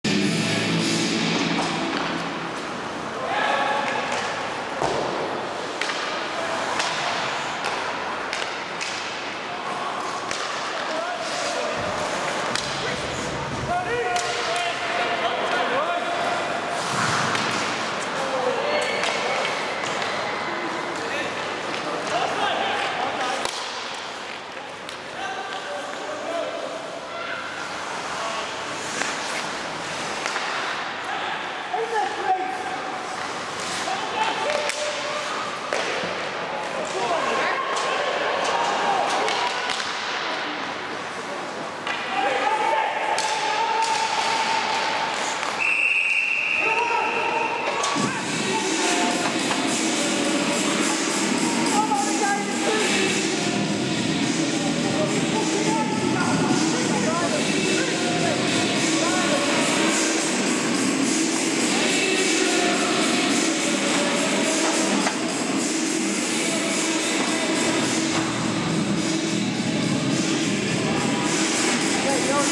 20130316 Coventry ENL_0059
Trafford Metros vs. Coventry Blaze ENL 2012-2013 Ice Hockey League Game Altrincham Manchester England Canon Legria HF S21